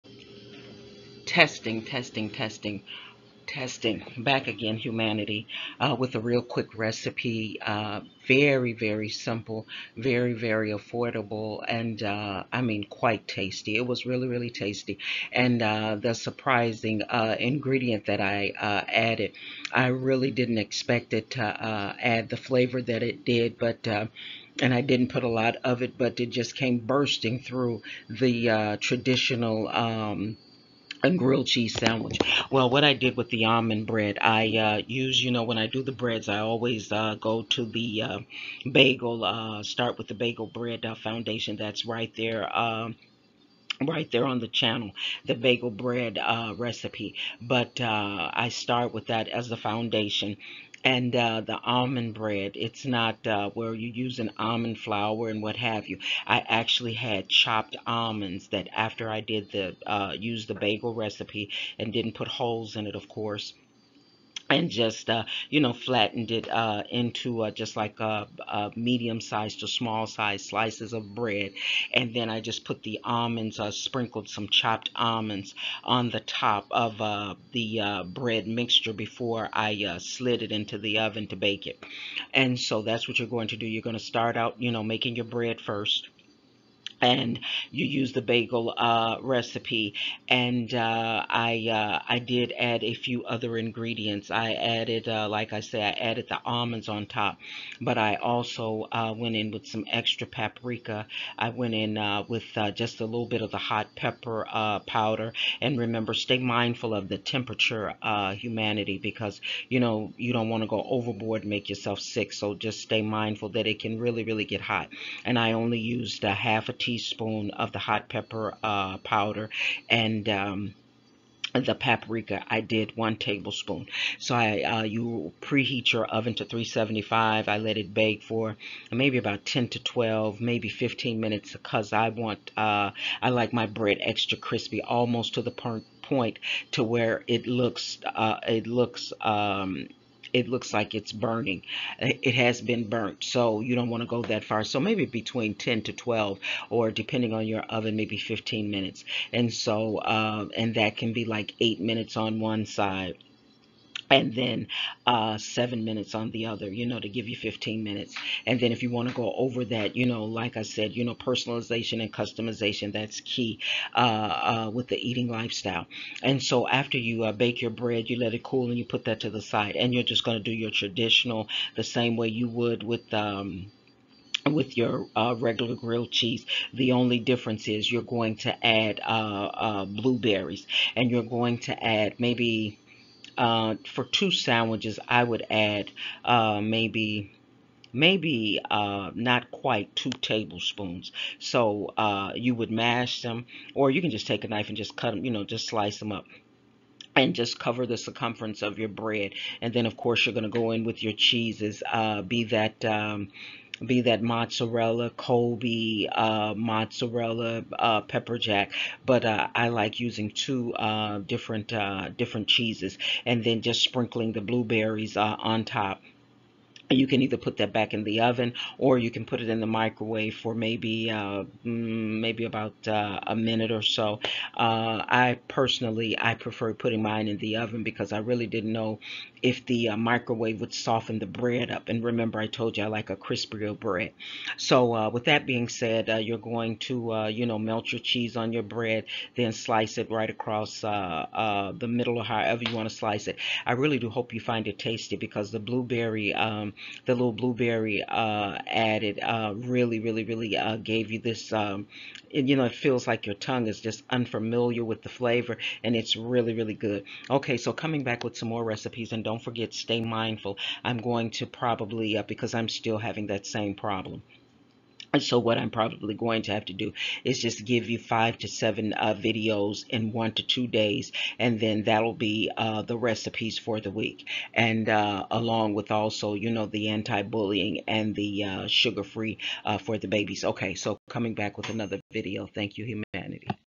HOMEMADE ALMOND BREAD AND BLUEBERRY GRILLED CHEESE
Another nutrition snack with no sugar and no salt.